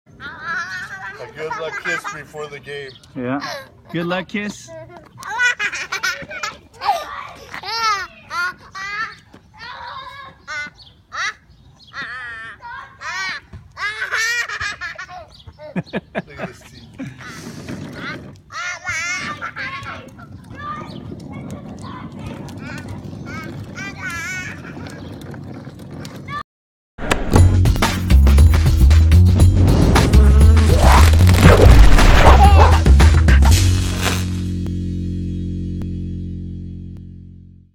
Newfoundland gives good luck kisses before big game
Before his baseball game, this little boy receives some good luck smooches from his giant Newfoundland dog. Now he'll be ready for game time!